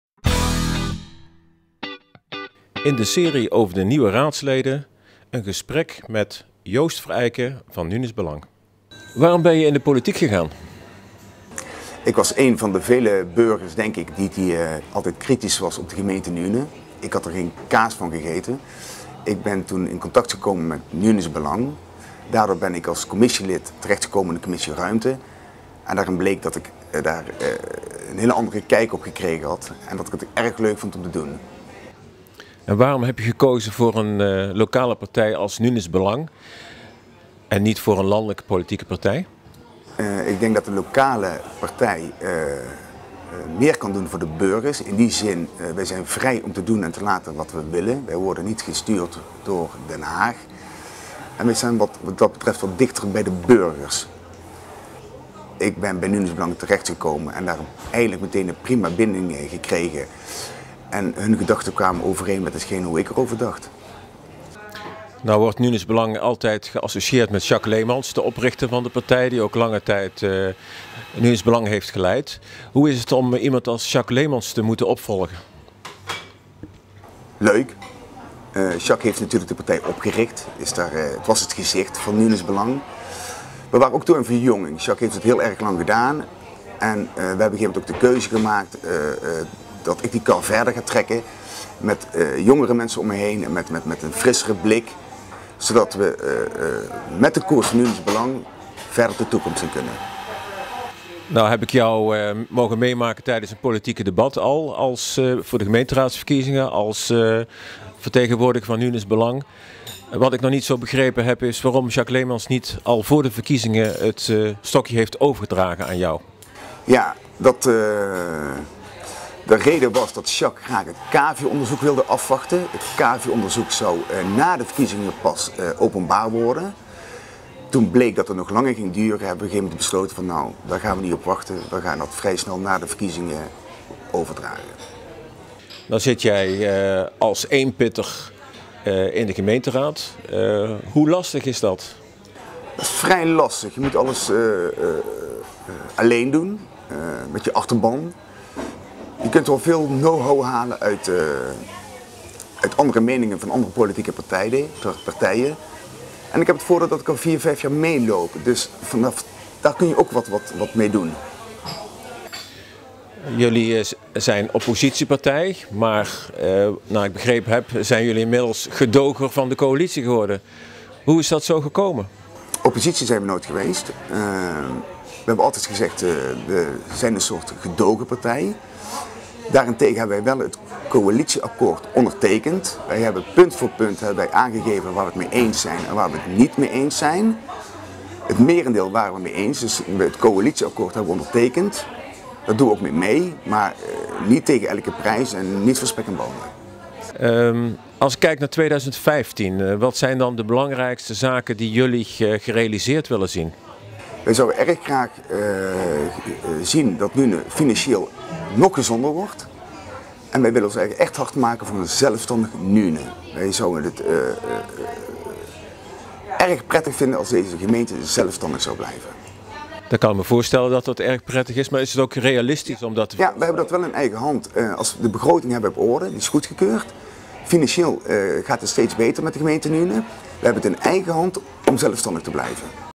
Joost Vereijken
Joost Vereijken vertelt waarom hij in de politiek is gegaan en waarom juist voor de lokale partij Nuenens Belang.